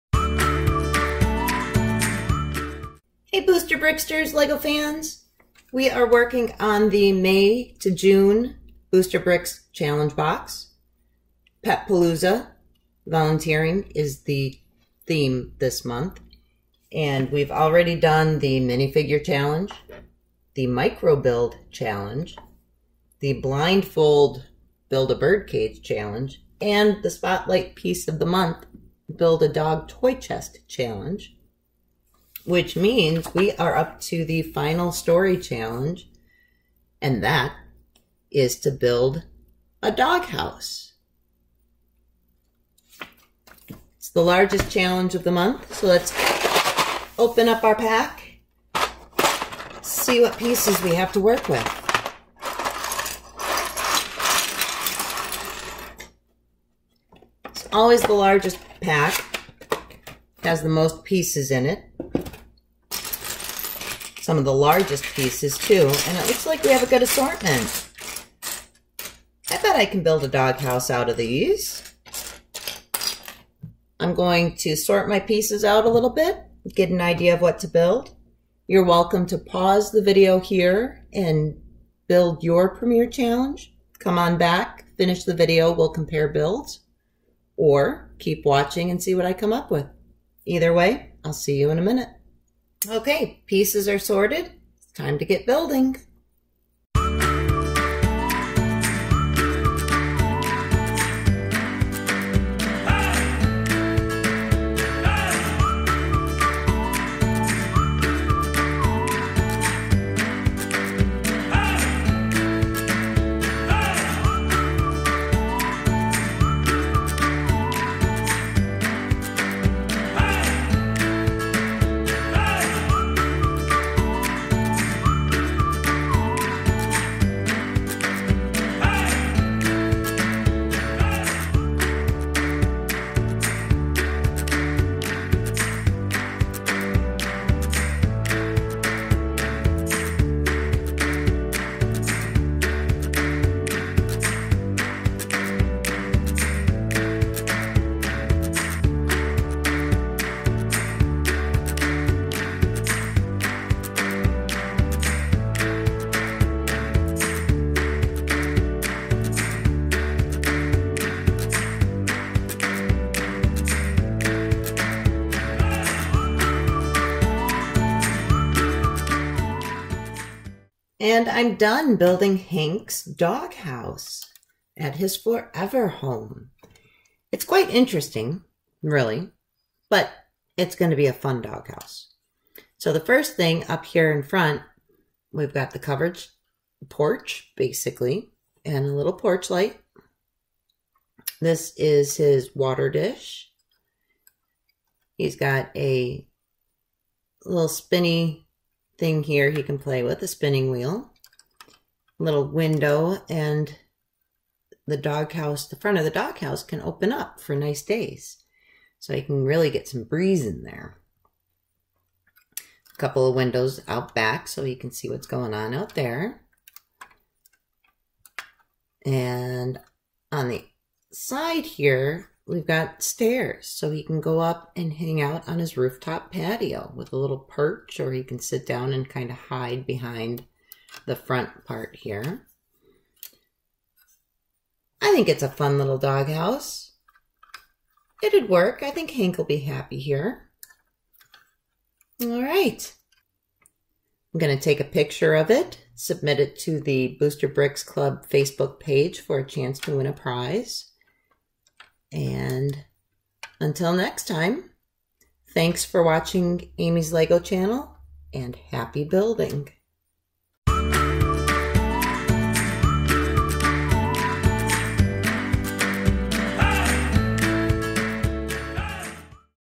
PREMIER Challenge - Booster Bricks - May-June 2019 Box
*** Links to all the build challenges below *** Unboxing and building the PREMIER CHALLENGE from the BoosterBricks May-June 2019 Challenge Box. The challenge is to build a dog house! Once complete, I'll post a pic of my build in the Booster Bricks Club Facebook group for a chance to win an awesome LEGO prize pack!